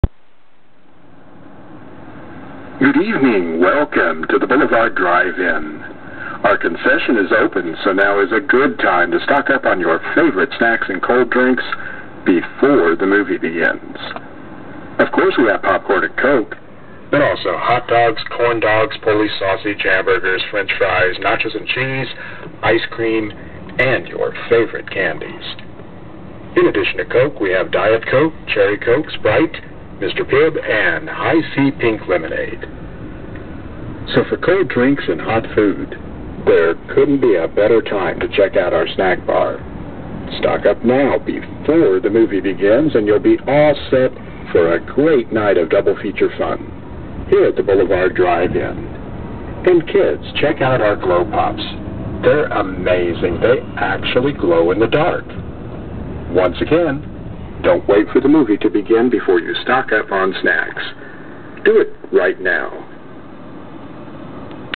Boulevard Drive In speaker
pre-show comments with drive in movie tour.